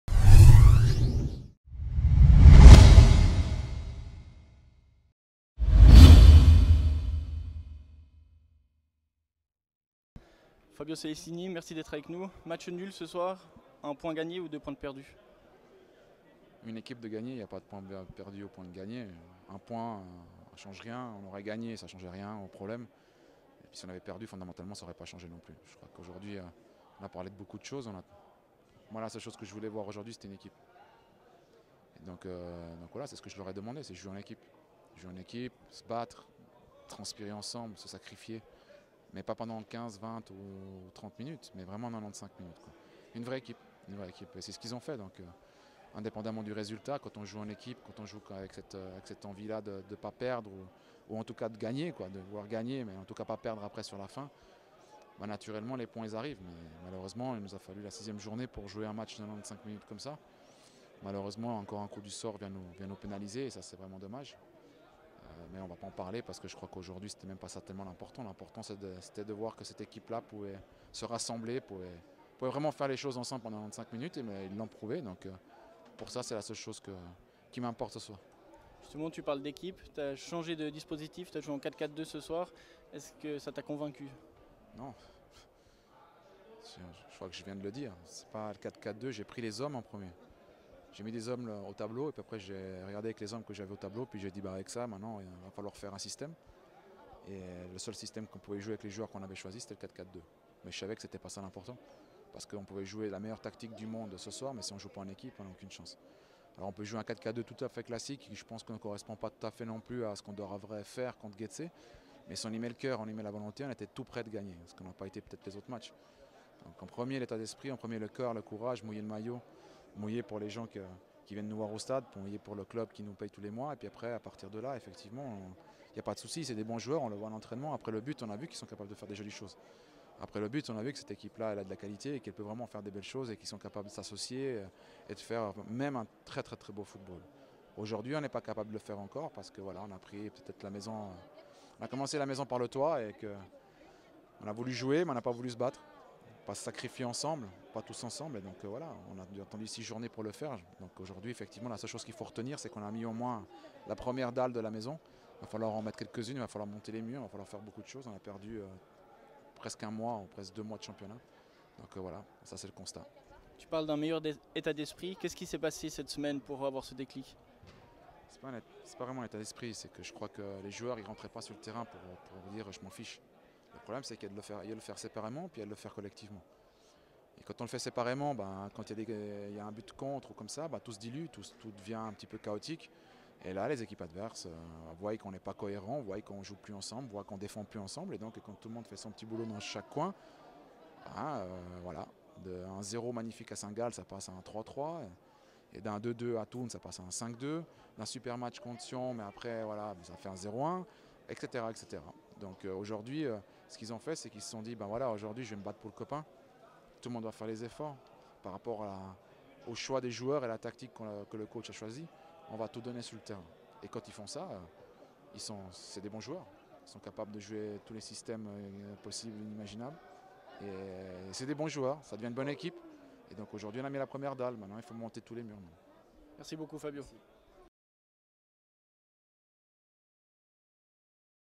LSTV: Lausanne Sport - Grasshopper Club Zurich, interview d'après-match avec Fabio Celestini
Fabio Celestini partage son analyse au micro de la LSTV après le match de Raiffeisen Super League entre le  FC Lausanne-Sport et le Grasshopper Club Zurich.